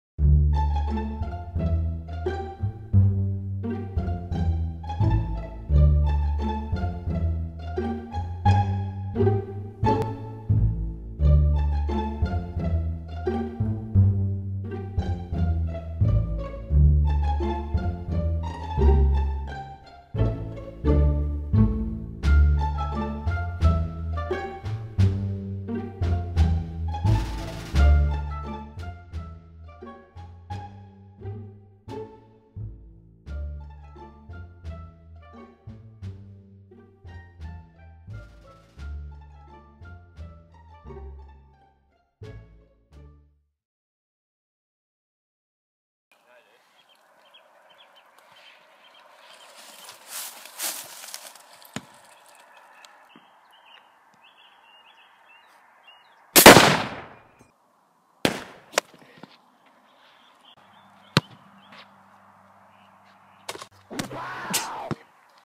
Footballer VS Russian Rifleman
When a pro Footballer is out practicing football tricks in rural New zealand. A random russian Rifleman spots him and takes his shot.